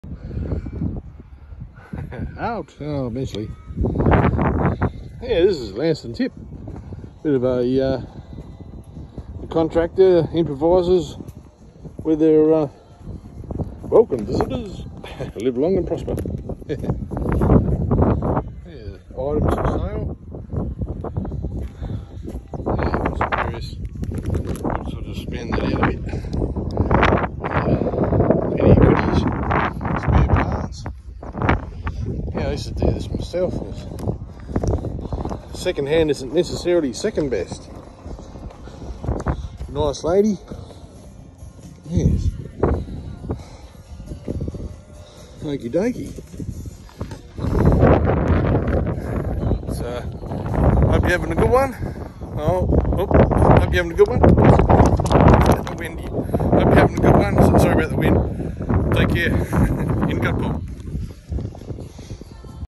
✌️Ang pagbisita kahapon sa lokal na tip, kung saan ang recycling ate repurposing ay nagpapakita ng »
» kanilang, Totoo Nagkakahalaga ‘Real Worth’, kasama ang huli, kumukuha ng unang Premyo 🥇✨ 🏆 📸 ✨🙌🥳 🎈